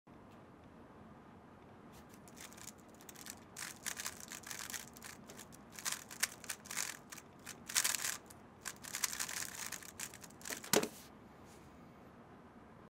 cube but timed
The GAN Smart Timer is an upcoming Bluetooth timer designed by GAN. This timer has a variety of cool features made specially for speedcubing:

• Buttons freeze until 1 second elapses to prevent accidental timer stops
• Compatible with GAN smart cubes and Cube Station app
• Records averages of 5
• Durable, high quality buttons

On pre-order at TheCubicle.com today! If you’re in the mood to get the new GAN 356 i Carry, check out our GAN Bluetooth Bundle